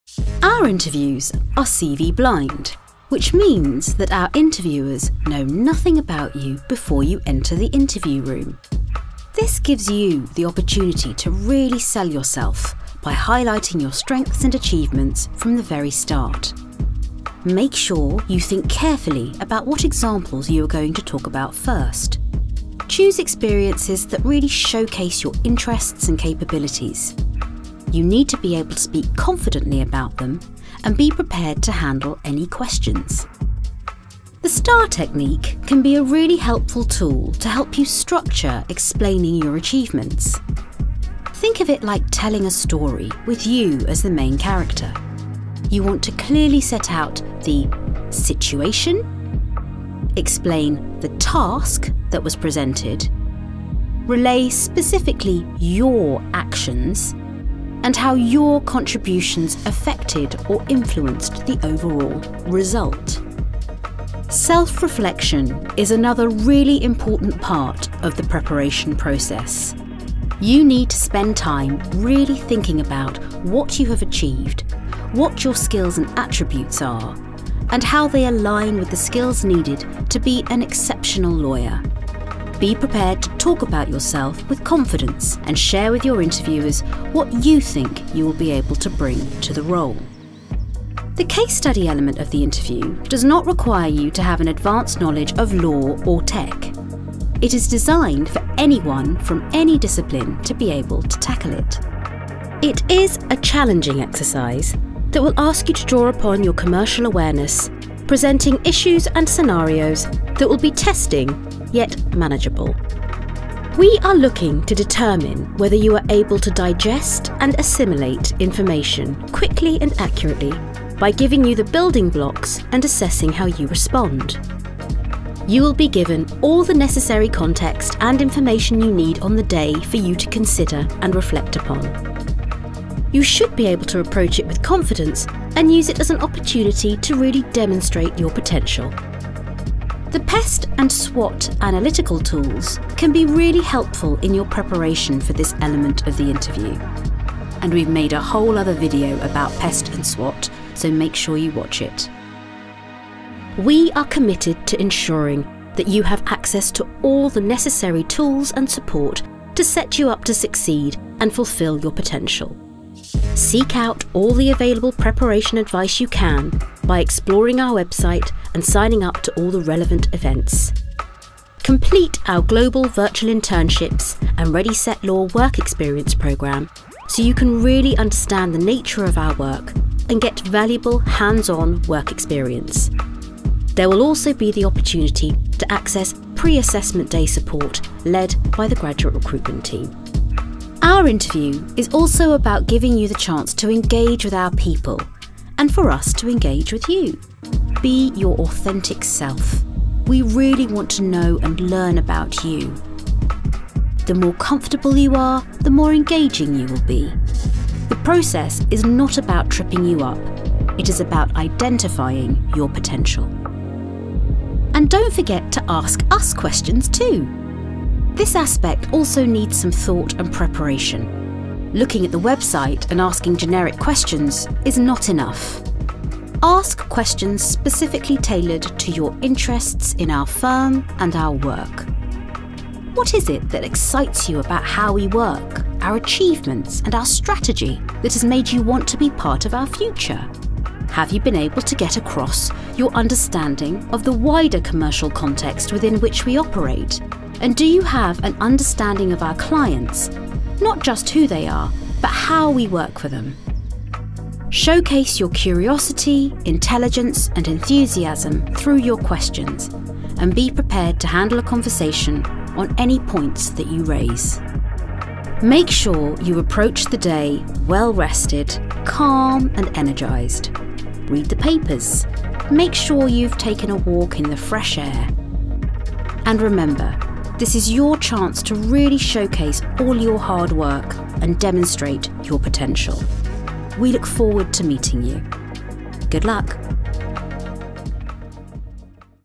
Clifford Chance London Graduates: Preparing for Interviews
Our London interviews require a lot of preparation not only for the case study element but also for the competency aspects. Here we take you through what to expect from the process, as well as provide you with essential advice on how to get ready to showcase your strengths and potential.